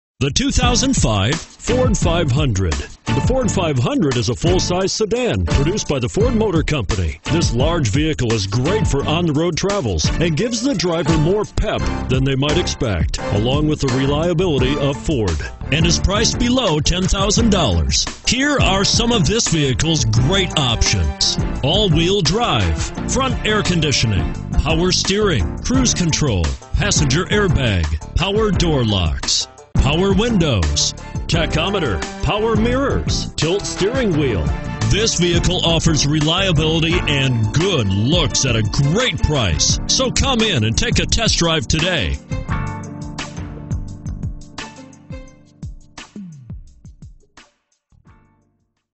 2005 Ford Five Hundred Carmel NY L0458
2005 Ford Five Hundred SE - $5500

**All Wheel Drive Air Conditioning : Power Windows :Power Locks :Anti Lock Brakes :** OIL & FILTER CHANGED ** ** CARFAX HISTORY AVAILABLE All used cars bought at Meadowland get a 100 point inspection ** Extended Service Plans Available for cars under 120,000 miles.